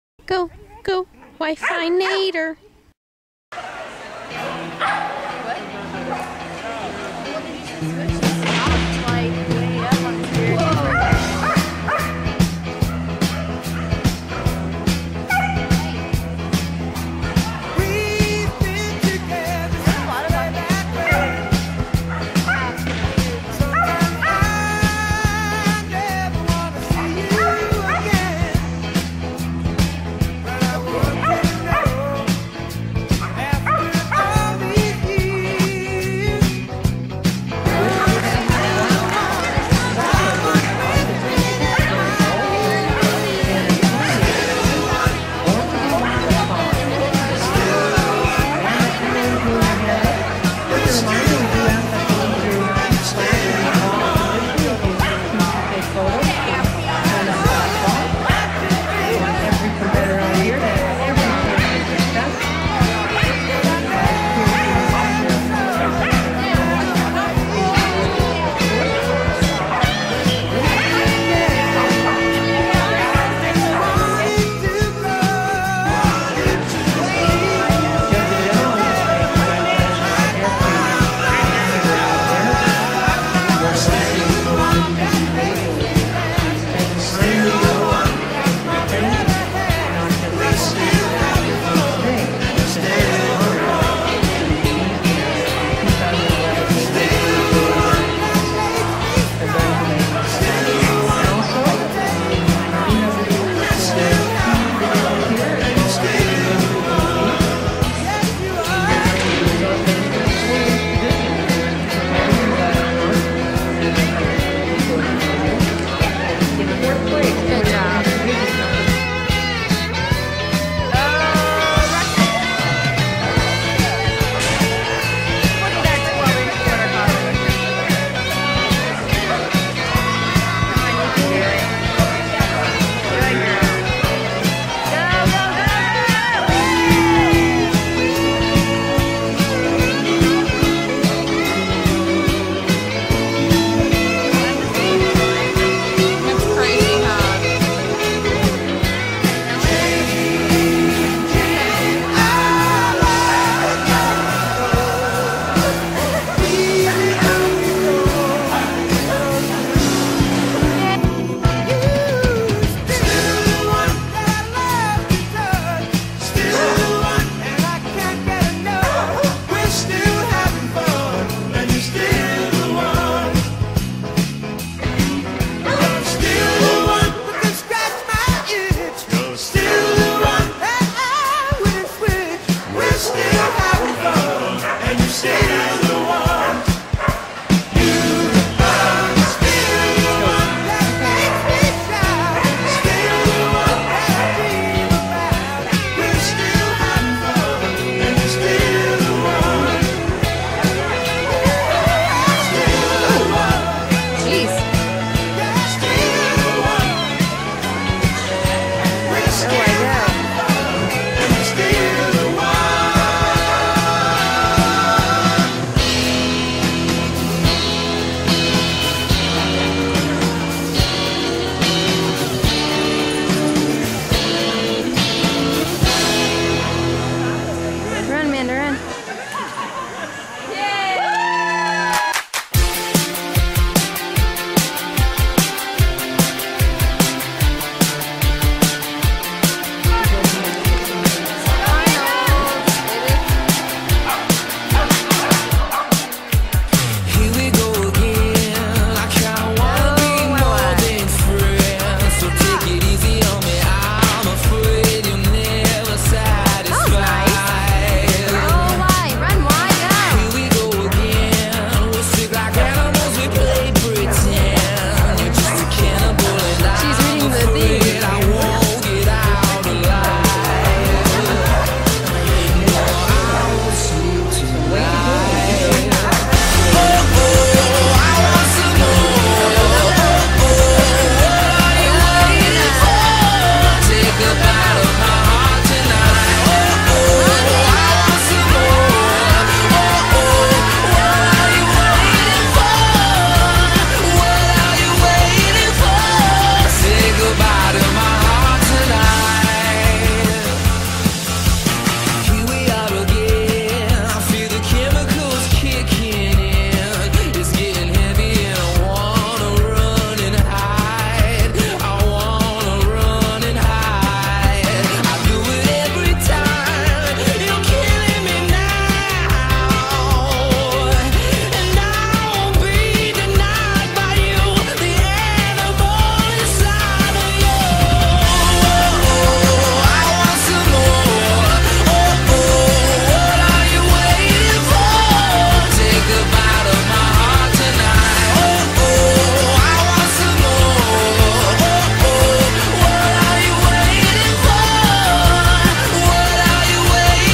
WiFi AKC Nationals 2011
Best. Weekend. Ever.
I love this dog to the moon and back. I could have never hoped for a better dog. WiFi really does rock.
I'm glad I waited a little while to make this video, because if I hadn't, I wouldn't have found the perfect song. :) Even though I recently got a puppy, WiFi still is, and always will be THE one. I hope she realizes that soon, since she still feels a little left out since the puppy came home.
I love you WiFi! Best dog ever.
By the way, YAY 4TH PLACE!